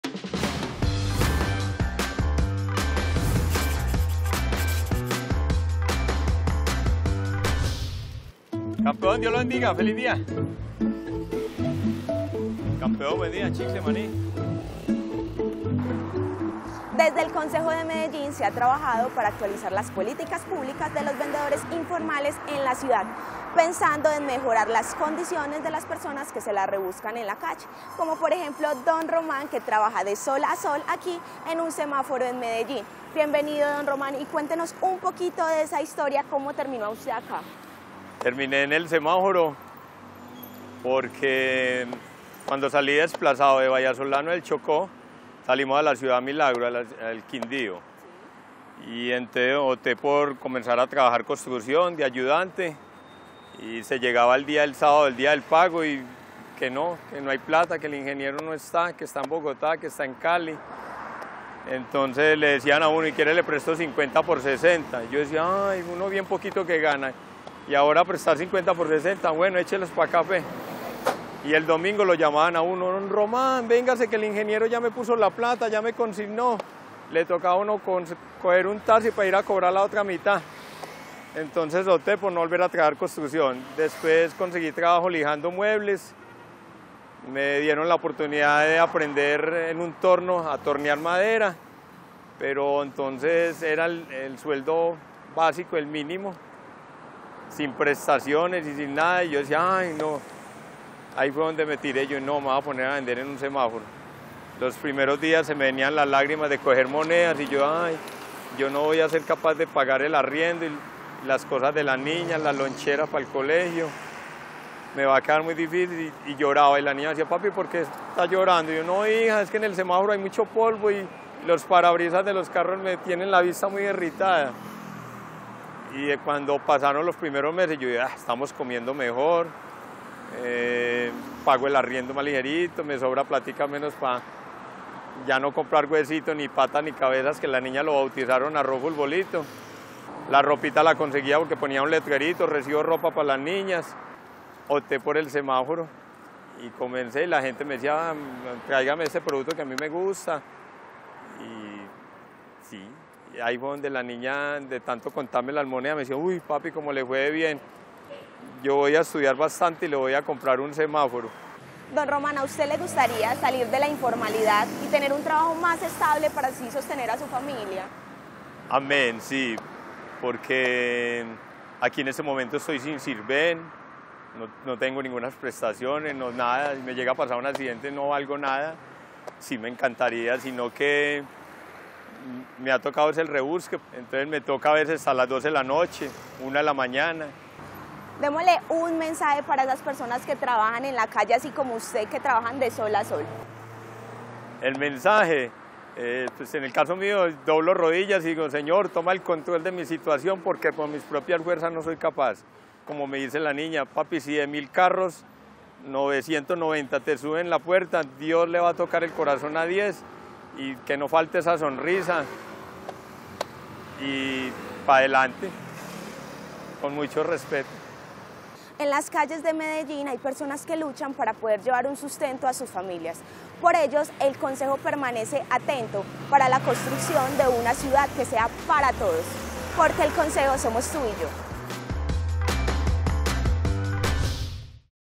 La historia de un vendedor informal
Román Sierra es ejemplo de muchos de los casos de vendedores informales que hay en Medellín y que son preocupación constante de las políticas de protección y formalización que impulsa en el Concejo del distrito. . Su caso, se repite en semáforos y esquinas de nuestra calles